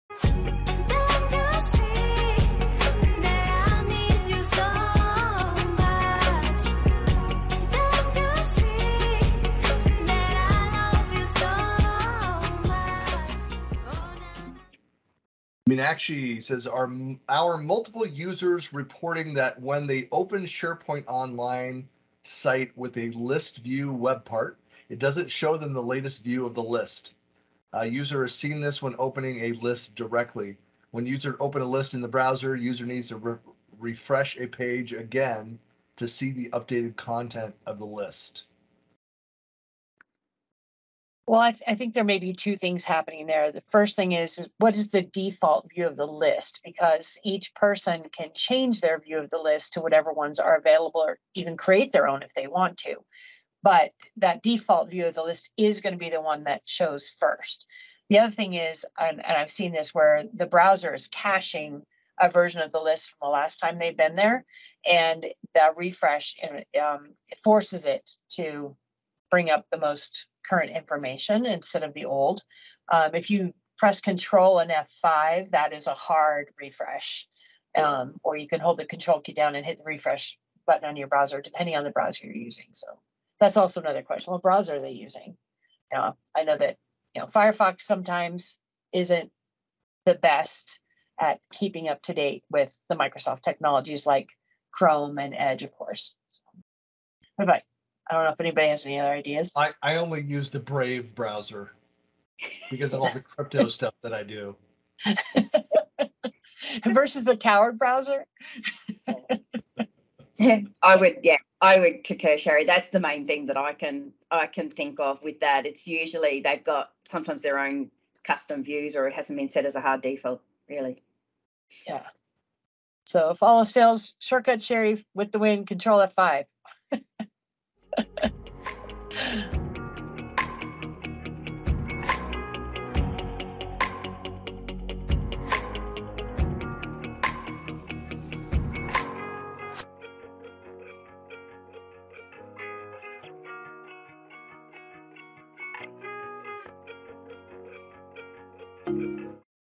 M365AMA Will a SharePoint List dynamically refresh in the browser?
In this episode, the panel discusses the following community question:

“Our multiple users reporting that when they open sharepoint online site with a list view webpart, it doesn't show them the latest view of the list. User has seen this when opening a list directly. When user open a list in browser, user needs to refresh a page again to see the updated contents of the list. Need your advice…"

Some relevant notes/links shared by the team:

-- Is it the default view of the list? Otherwise it may be browser cache issue.